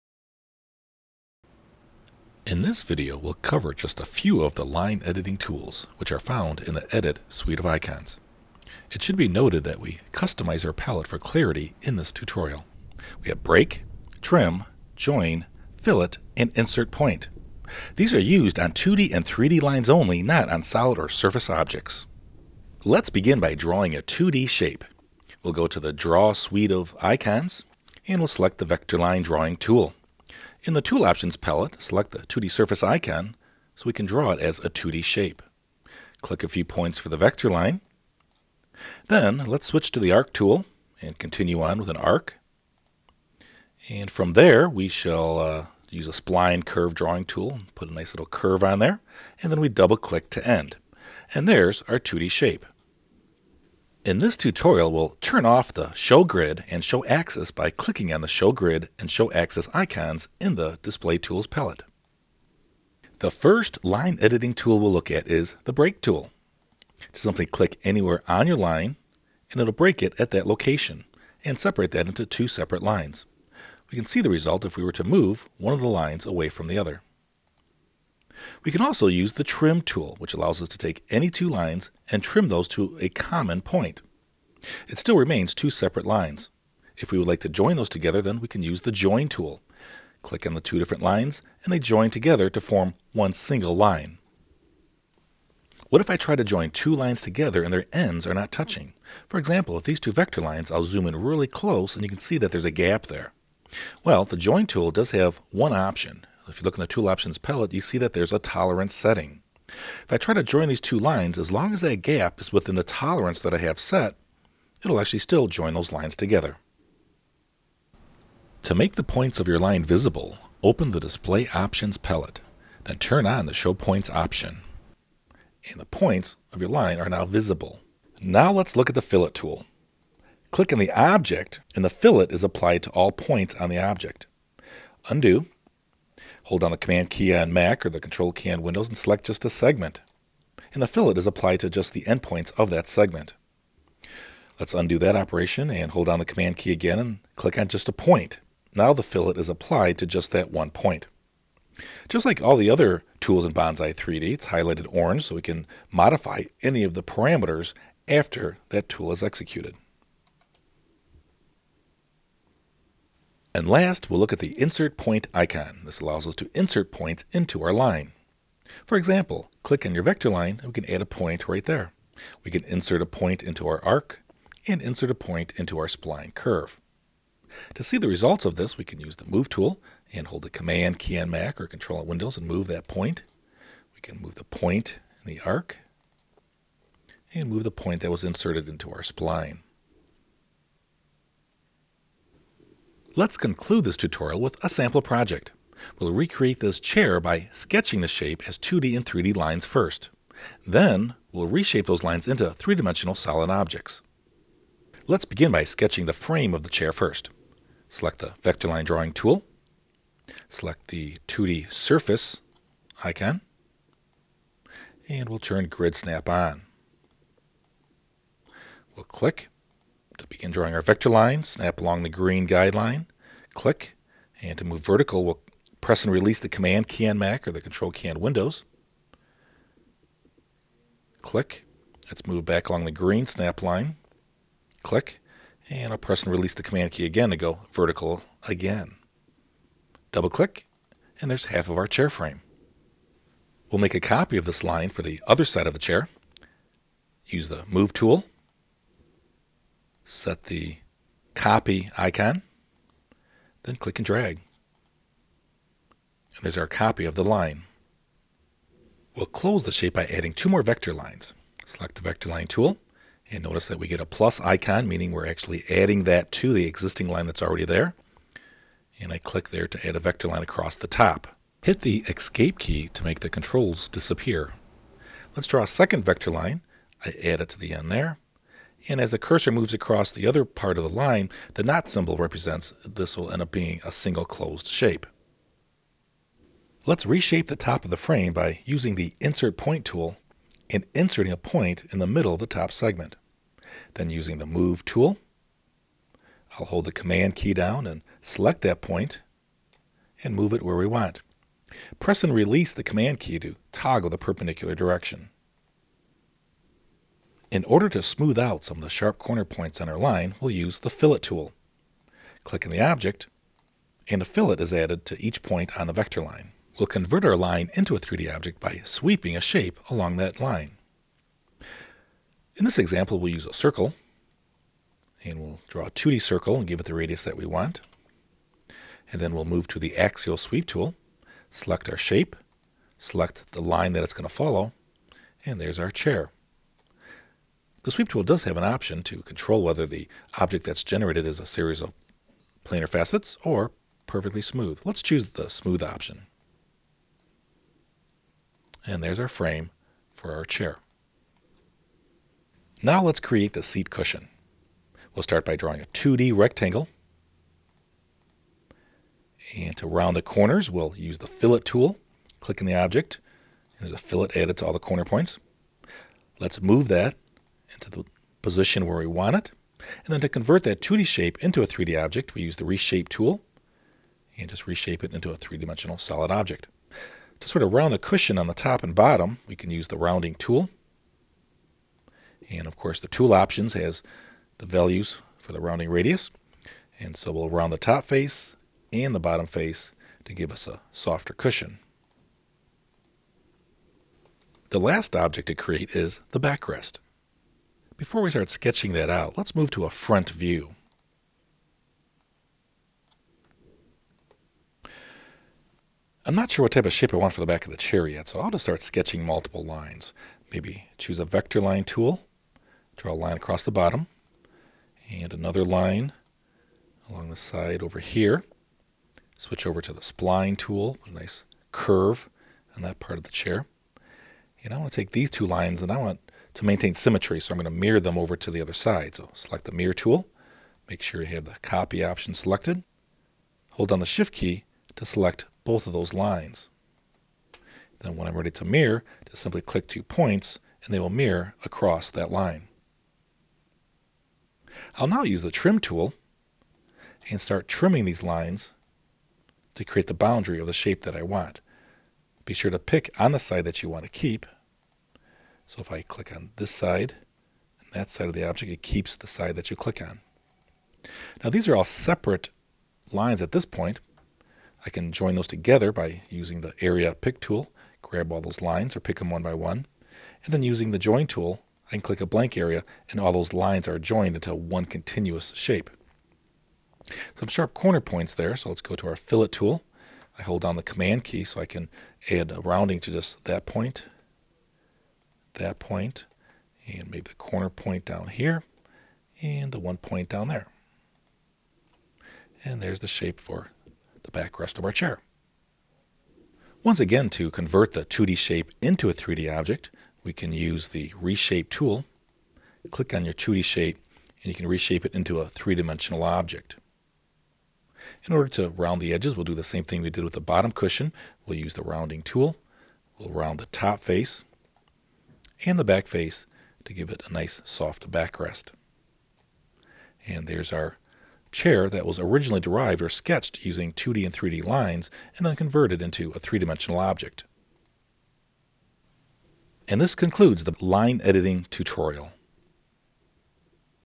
formZ Line Editing
See how the Break, Trim, Join, and Fillet tools contain all the functionality you need for editing 2D and 3D lines.
R2